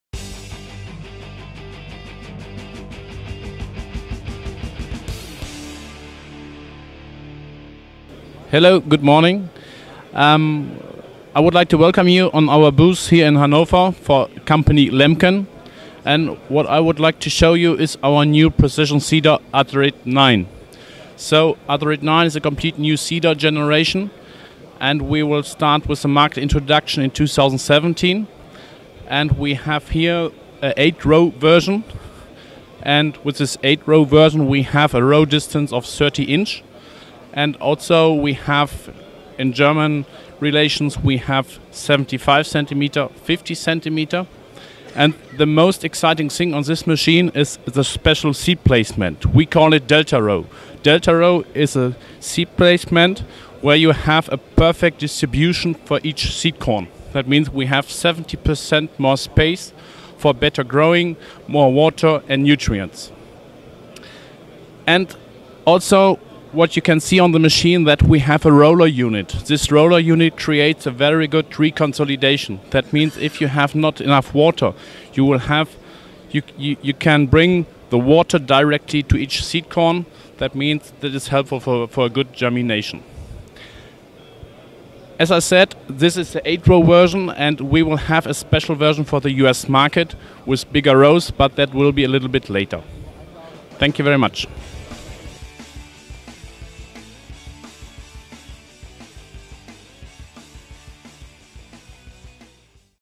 Lemken Unveils New Precision Planter: Azurit 9
Ralf Bornemann, product manager for Lemken, introduces the company's Azurit 9 precision planter at Agritechnica 2015. DeltaRow differentiates Lemken technology from traditional approaches, in which seeds are placed in single rows. The Lemken designers have instead opted for two staggered twin rows spaced about 5 inches apart. This gives individual plants 70% more surface area for growth and ensures better access to water and nutrients.

The Azurit allows corn, soybeans, sunflowers and rapeseed to be sown with great precision and efficiency at forward speeds of about 9.3 mph.